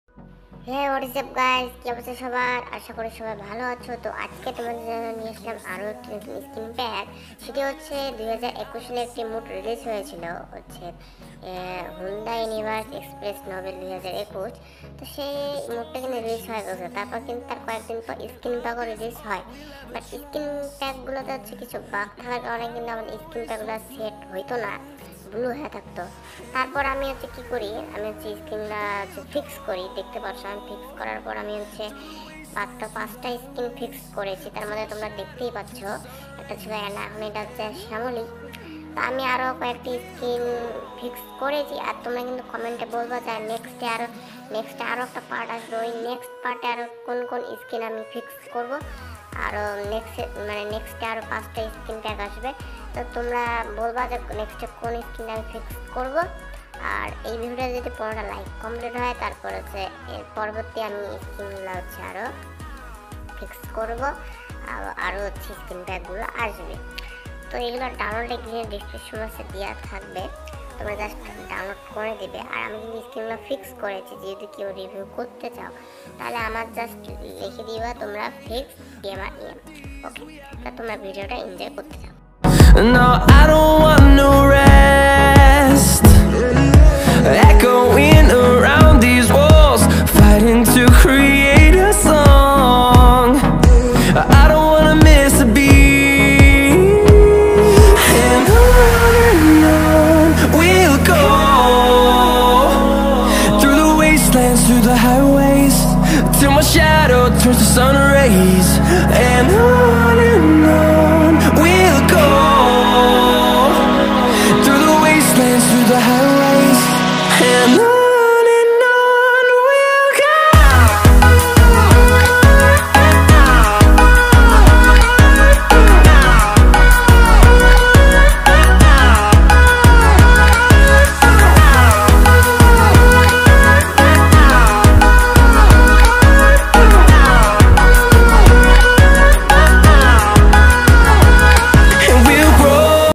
Top 5 skin bug fix for Hyundai universe express nobel 2021 mod। Download skin।
হেয় Friends আমি Eshan From Gamer EM আমি তোমাদের জন্য প্রতিনিয়ত স্কিন প্যাক নিয়ে আসি তো।।।।। জারা

cmnt এ বলো  Next এ কোন  মোড এর স্কিন আনব 😉😉😉

bug fix by Gamer EM (me)

Cmnt kore bolo next a kon Hyundai  bus er skin fix korbo

skins